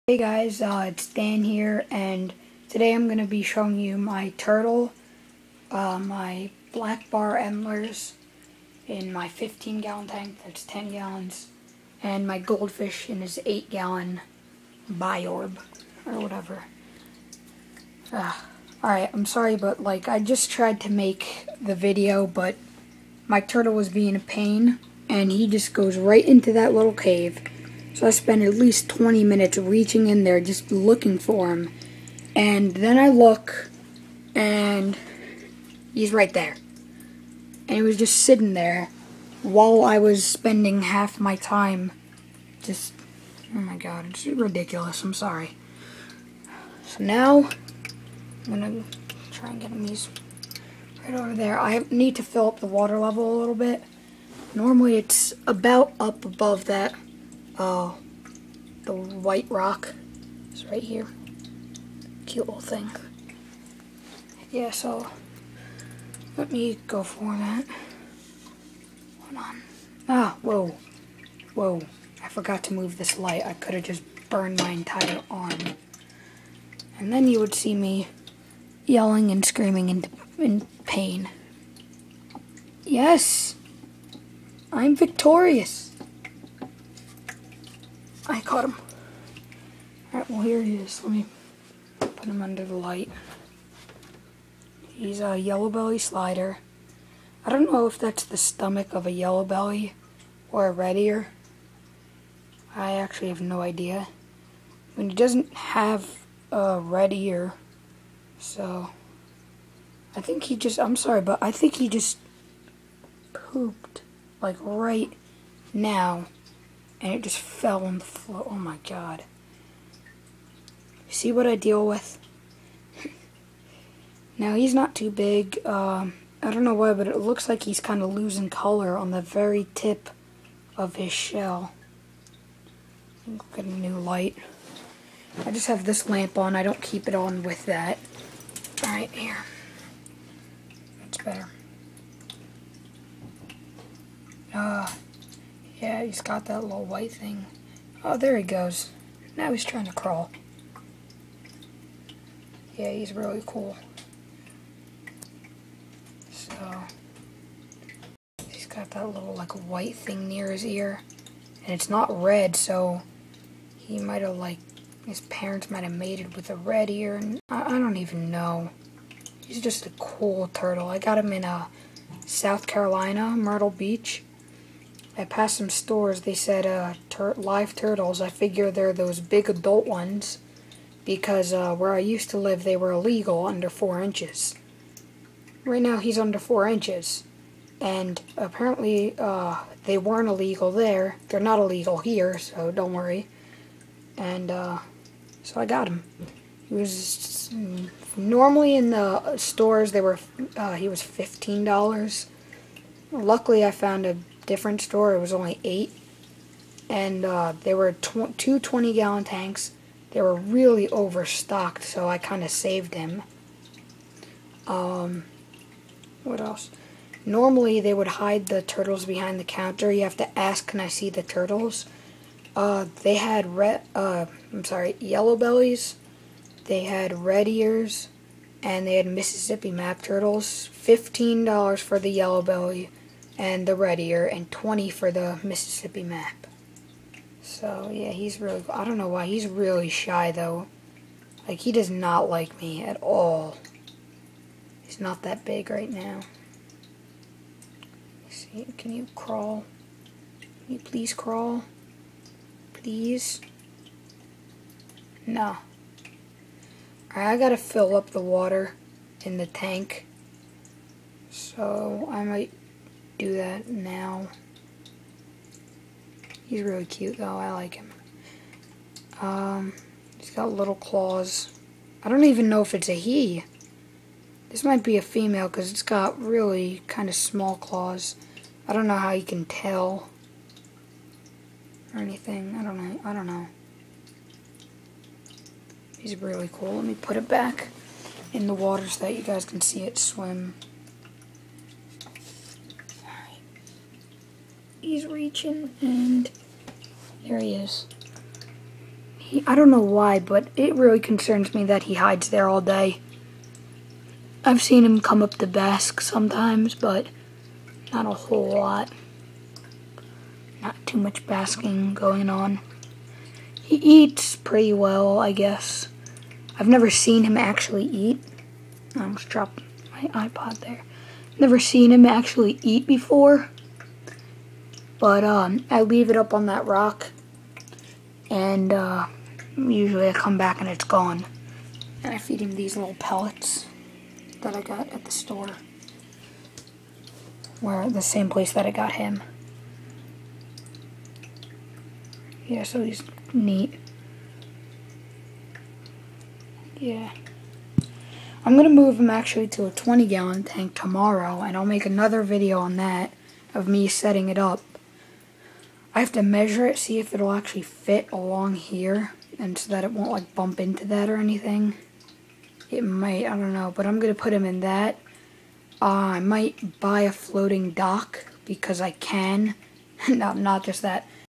Baby Yellow Belly Slider part 1
My yellow belly slider from Myrtle beach, SC in a ten gallon tank.  Any comments or questions just ask.  Requests for other video will be taken.